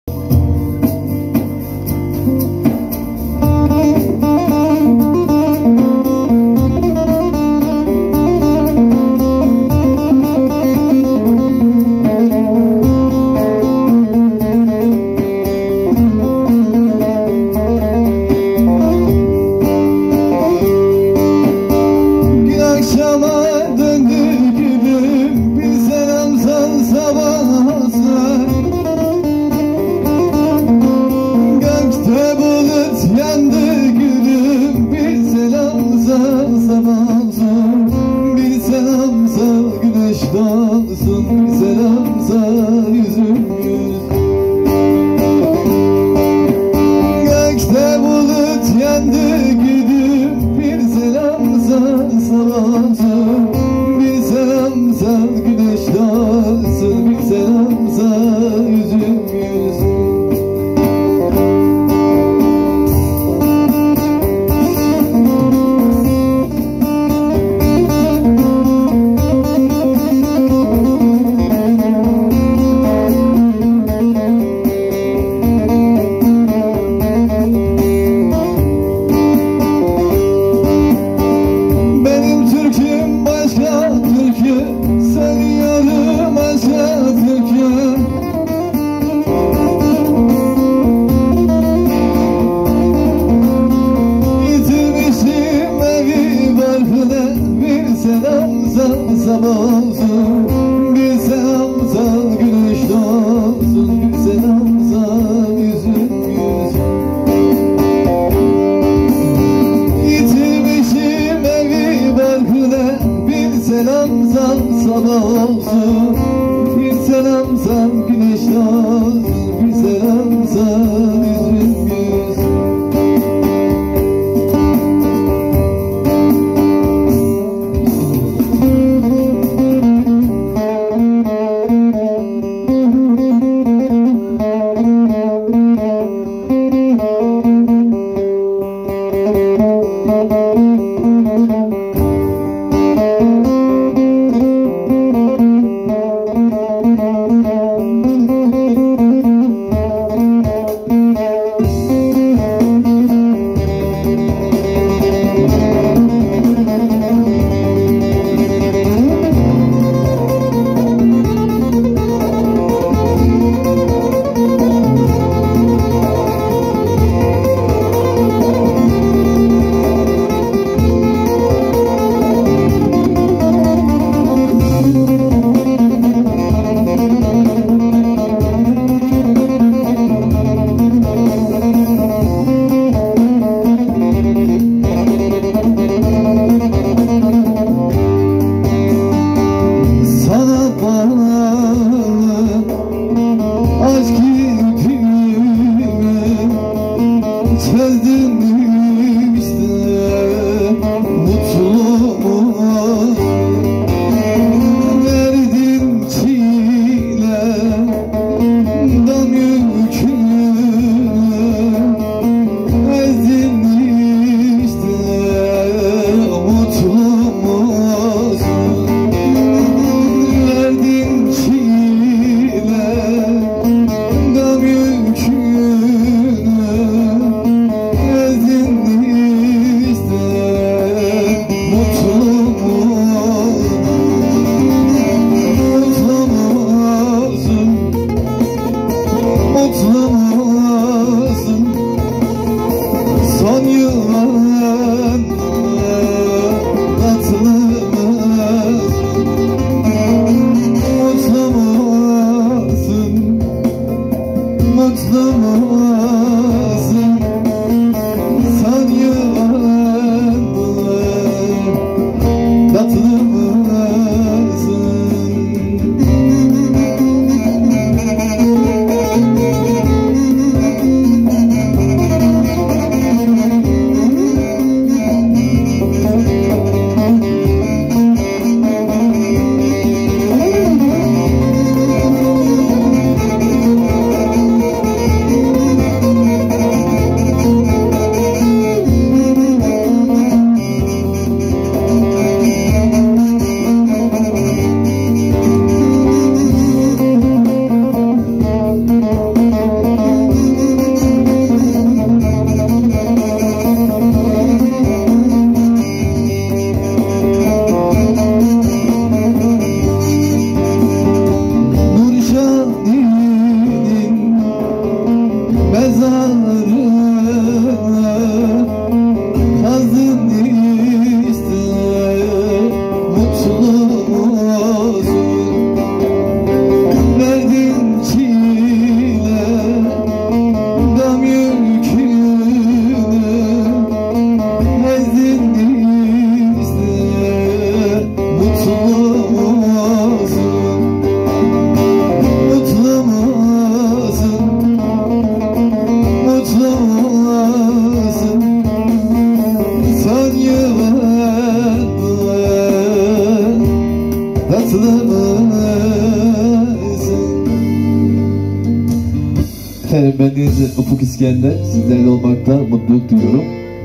UFUK İSKENDER BAYRAM ETKINLIKLERI SAHNEMIZ TÜRKÜLERLE
Bayram etkinlikleri sahnesinden güzel türküler sevenlerimize